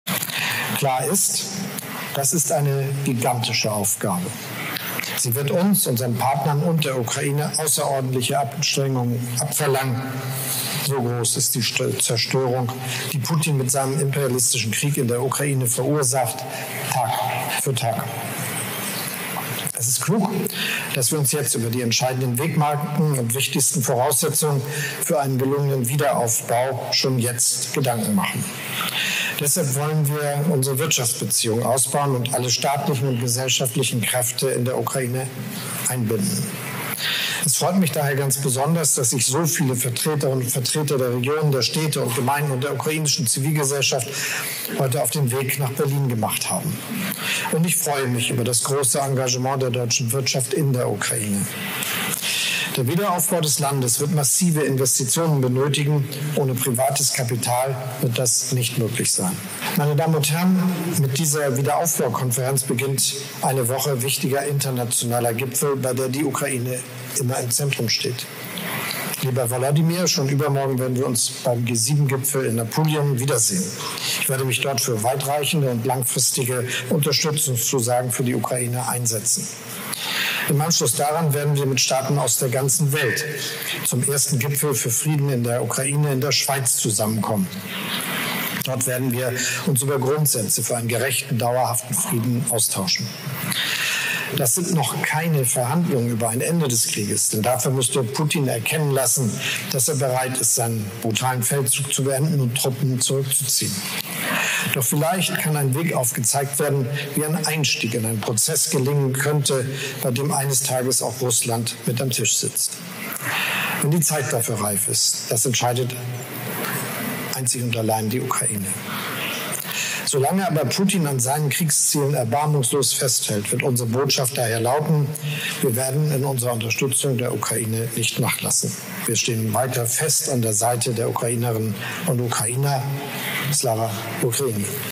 Wiederaufbau der Ukraine für Scholz eine „gigantische Aufgabe“
Bundeskanzler Olaf Scholz hat der Ukraine Deutschlands Unterstützung für den Wiederaufbau des Landes zugesichert. Scholz zeigte sich besonders erfreut über das Engagement der deutschen Privatwirtschaft in der Ukraine.

Alle Informationen zur aktuellen Lage in der Ukraine finden Sie in unserem Newsblog